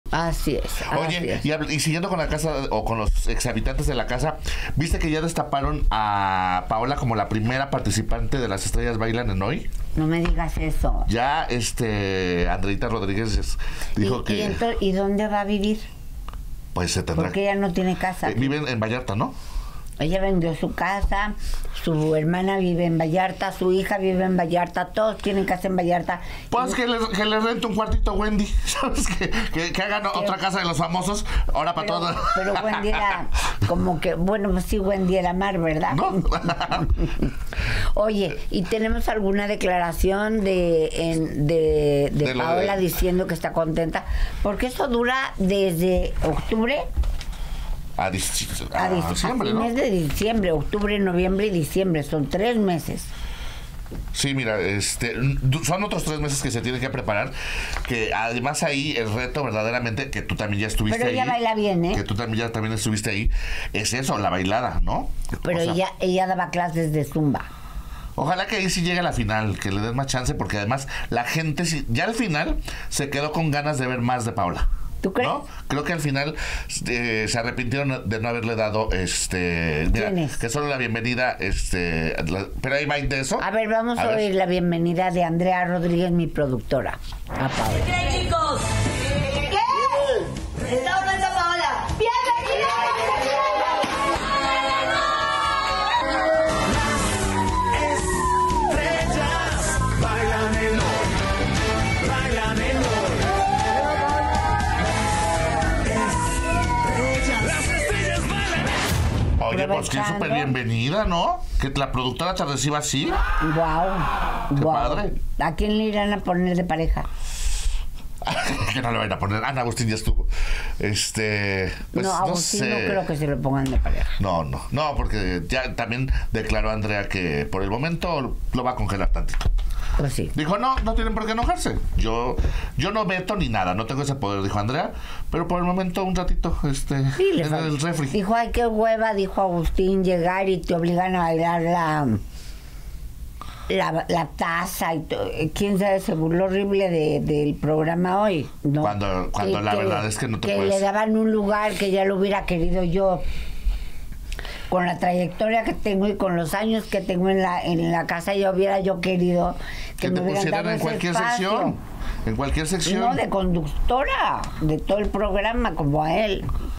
Paola Durante, primera confirmada en 'Las estrellas bailan en Hoy'
Paola Durante confirma su entrada a 'Las Estrellas bailan en Hoy', tras dar la noticia, se notó que el público aún quiere verla en la televisión.

Grupo Fórmula

Canal sugerido

Descarga nuestra